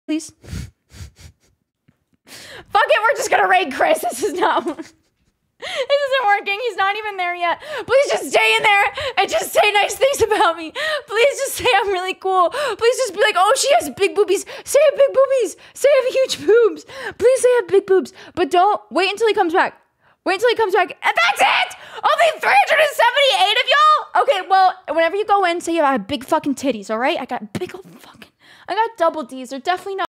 PeachJars- Say I Have Huge Boobies!
Peach breaks down while launching a raid. And she wants her viewers to send a very specific- and somewhat questionable- raid message.

and to judge for yourself how big her boobies are, check out her reddit & OF.